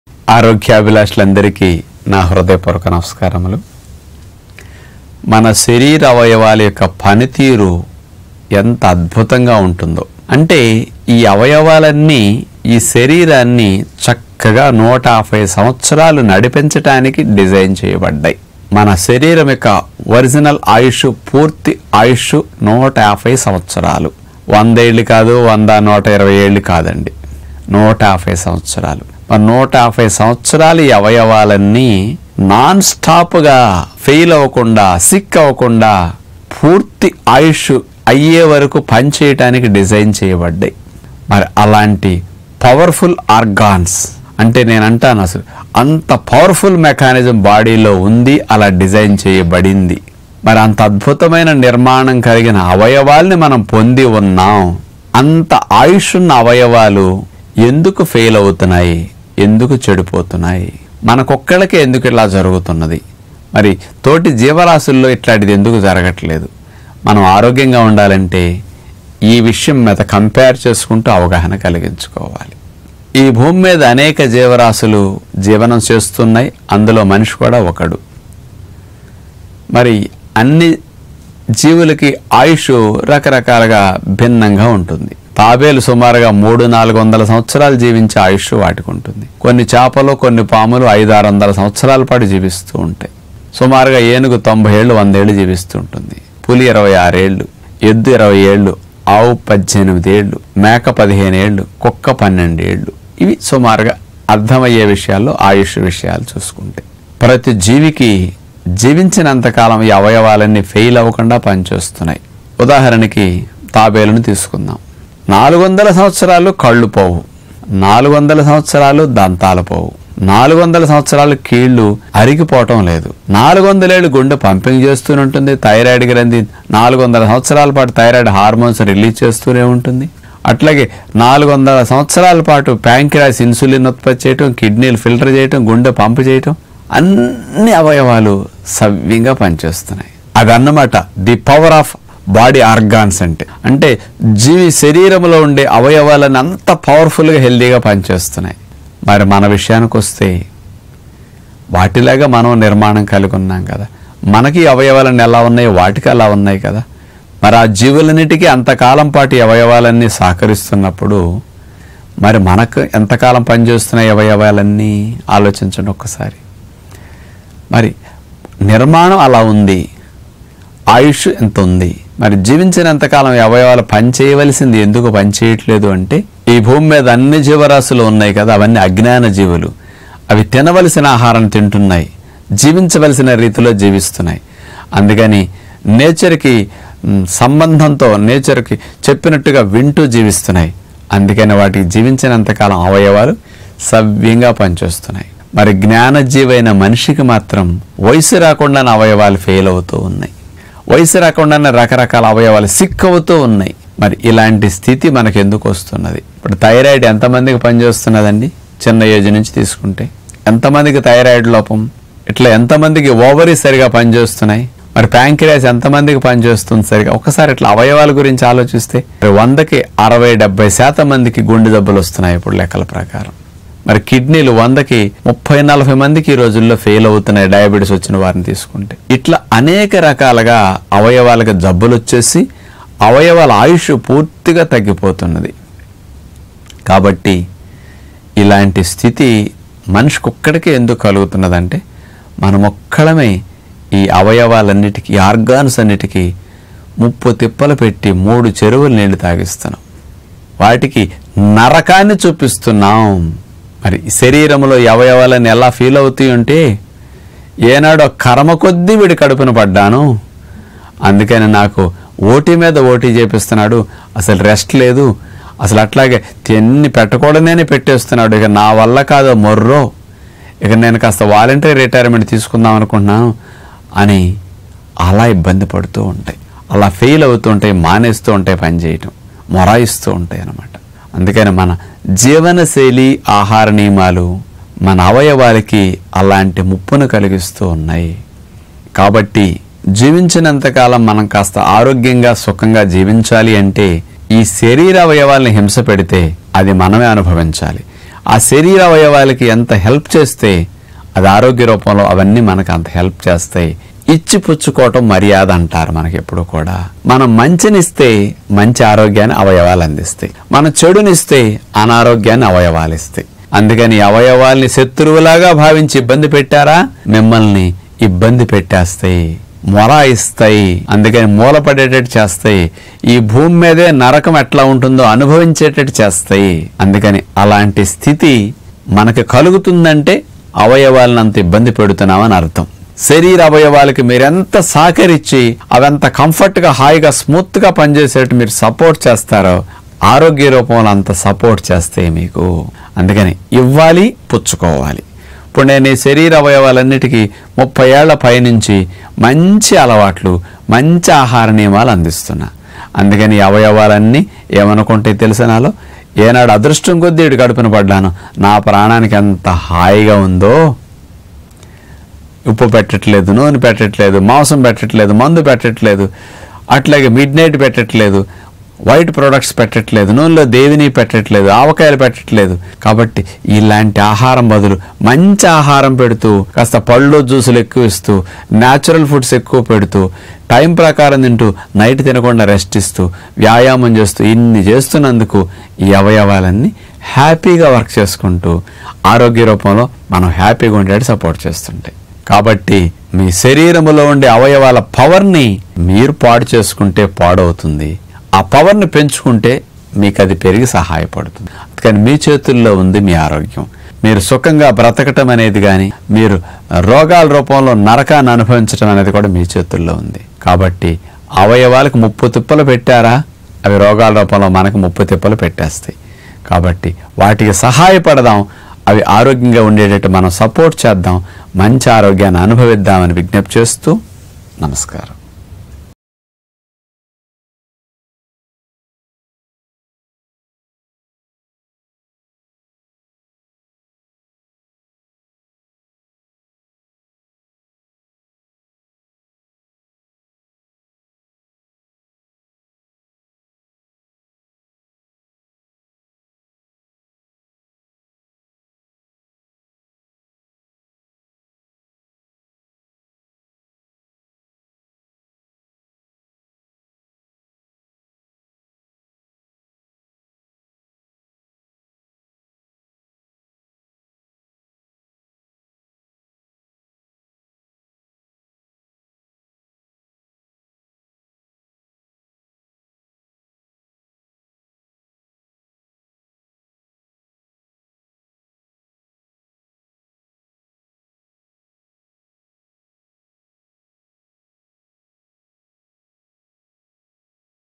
Power Of Organs 💪🦾💪🦾 మన శరీరంలో ఉన్న అవయవాల ఆయుష్షు ఎంతో తెలుసా ? | Dr Manthena Satyanarayana Raju
Power Of Organs 💪🦾💪🦾 మన శరీరంలో ఉన్న అవయవాల ఆయుష్షు ఎంతో తెలుసా ? | Dr Manthena Satyanarayana Raju | HEALTH MANTRA